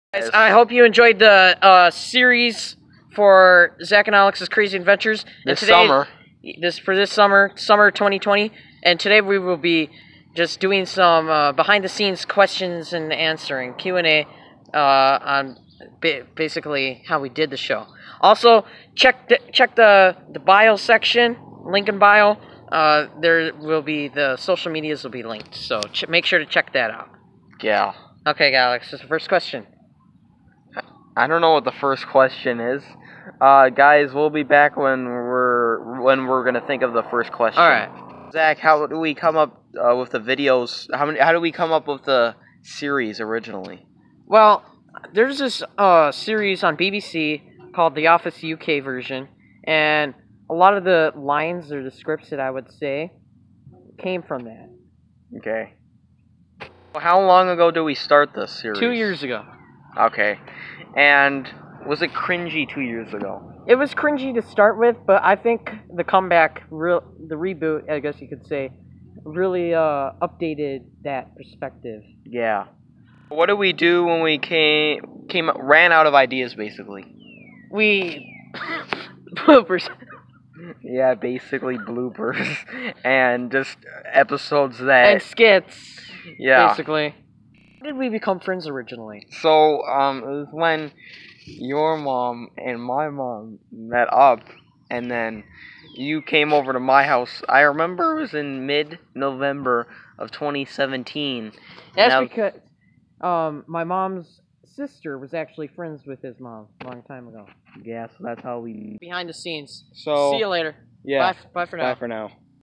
Z&A Official Q&A 2020
Today zach and I are telling you how we made the show.

I hope you enjoyed for this summer more coming next summer, we might have some holiday specials but thats it for now.